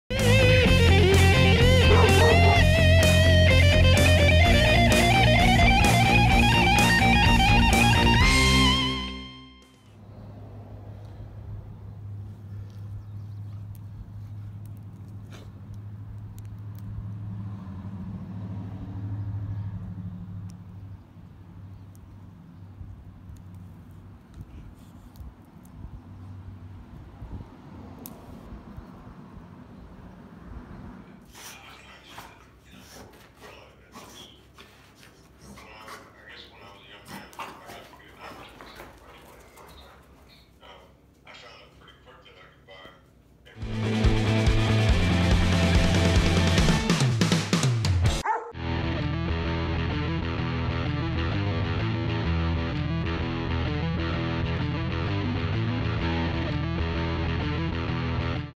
Rottweiler Body Language - Sticks and Rolling, how cute?!
Dog body language for a Rottweiler is especially important. Knowing the distinction between play and guard dog is important. Our goofballs love to play but would take a bullet for us. How about yours?